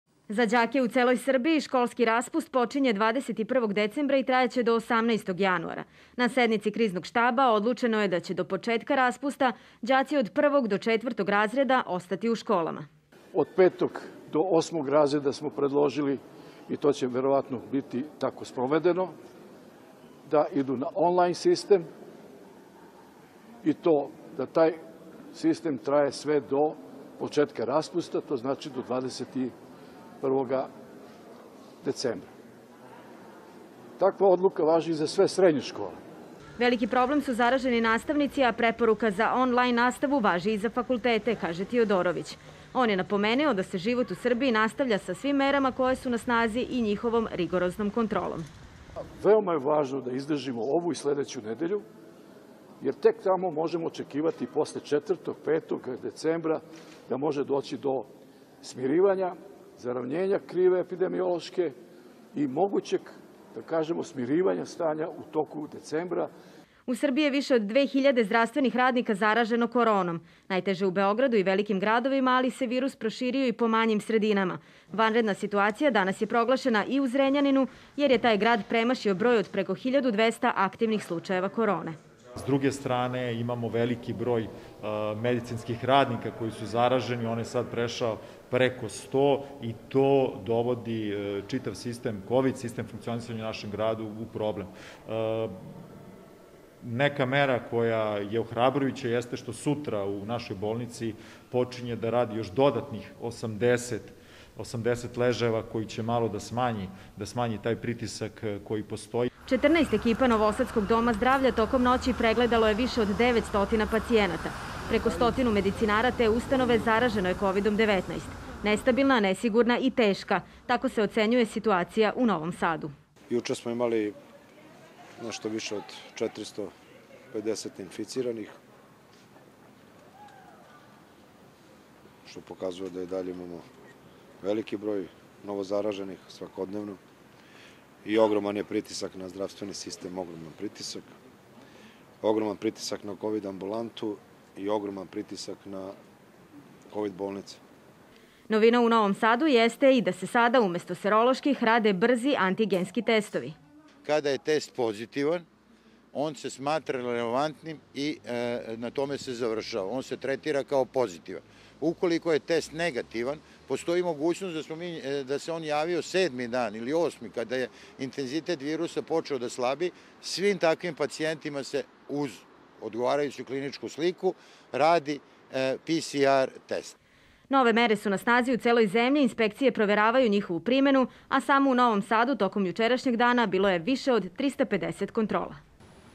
Krizni štab: Srednje i osnovne škole od petog do osmog razreda prelaze na onlajn nastavu
Školski raspust počeće 21. decembra i trajaće do 18. januara 2021. godine, a stariji razredi osnovnih i srednje škole trebalo bi da do tada pređu na onlajn nastavu, saopštio je danas epidemiolog Branislav Tiodorović posle sednice Kriznog štaba.